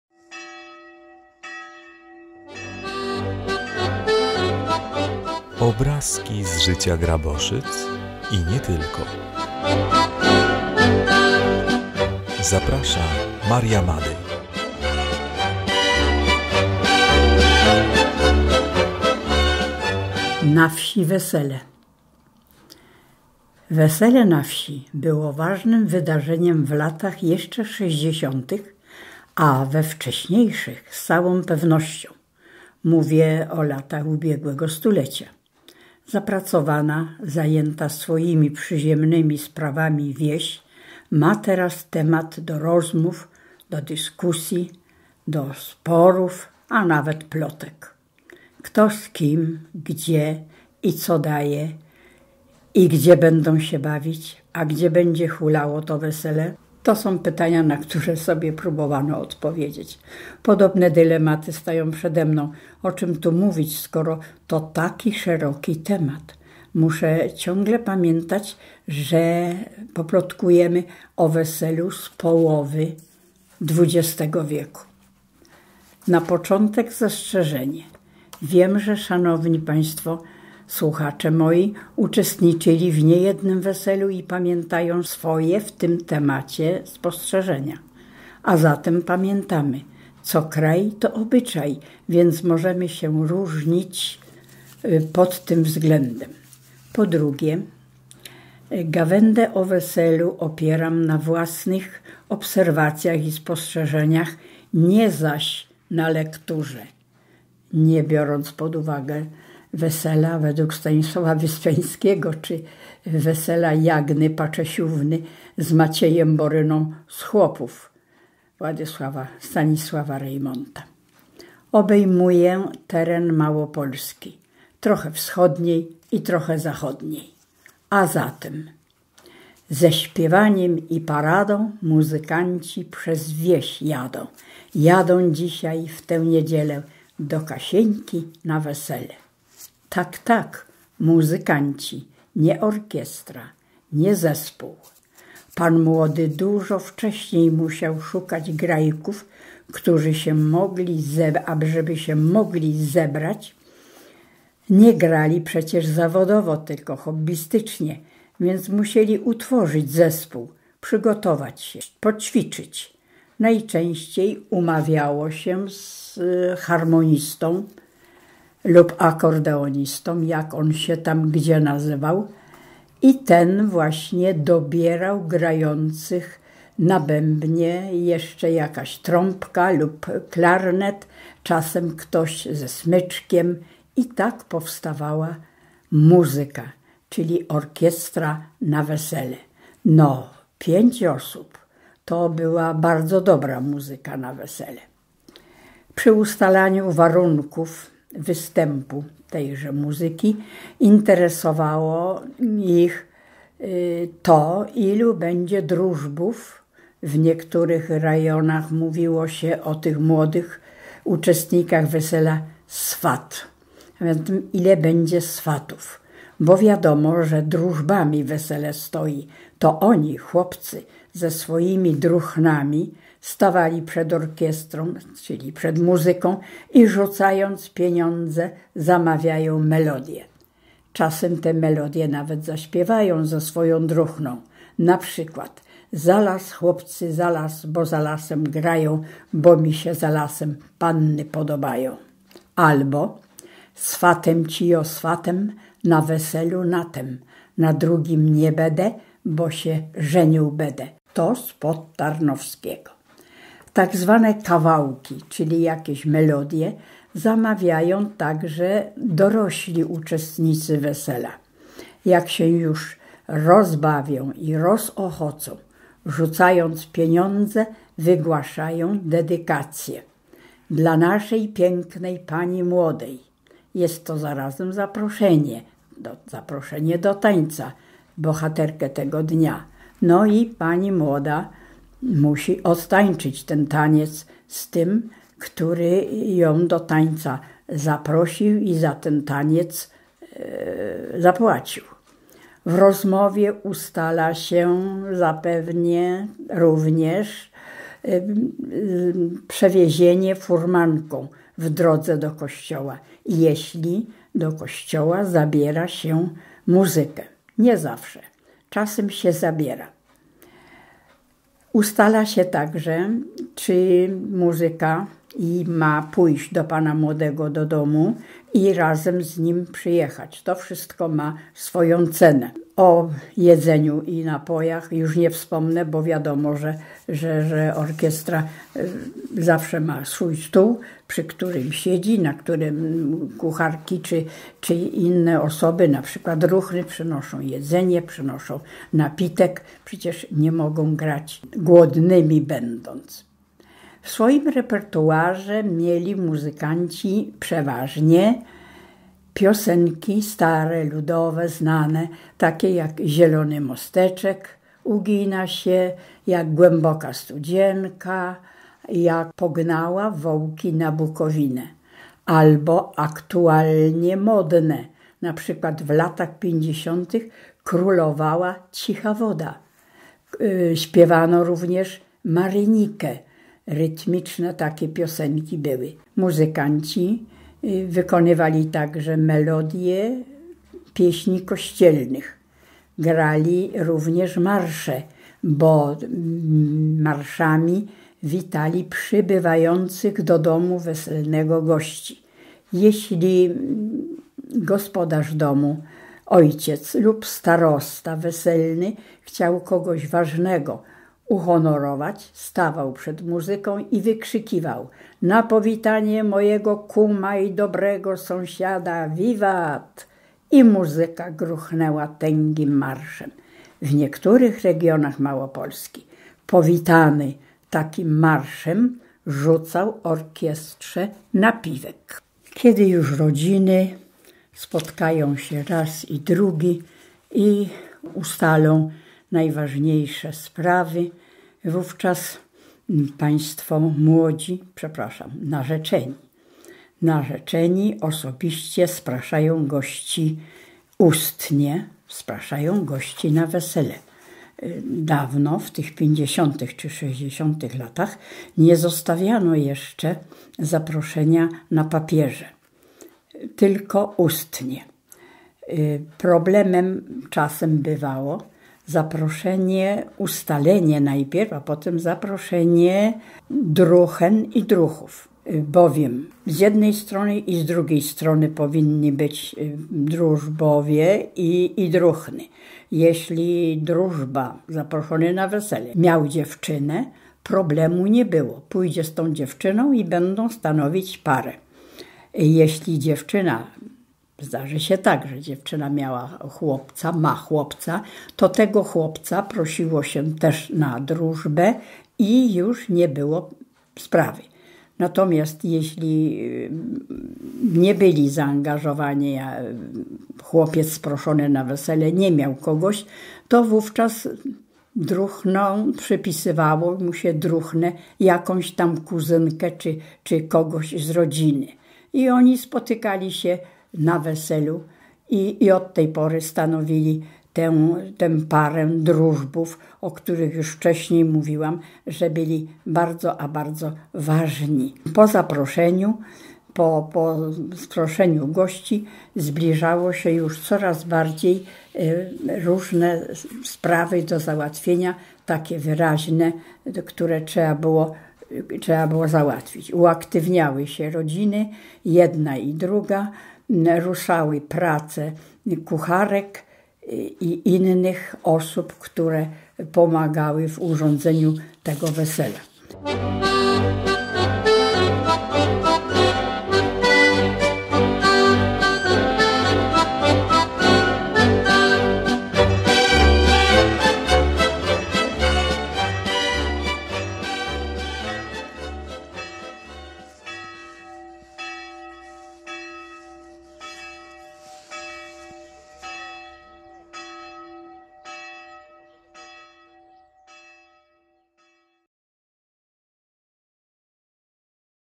Odcinek 18. Maria Madej. Obrazki z życia Graboszyc... i nie tylko. Wesele na wsi
Wesele na wsi - ważna rzecz. W pierwszym odcinku opowieści o weselach z połowy XX wieku Maria Madej opowie o muzykantach, zapraszaniu gości i drużbach.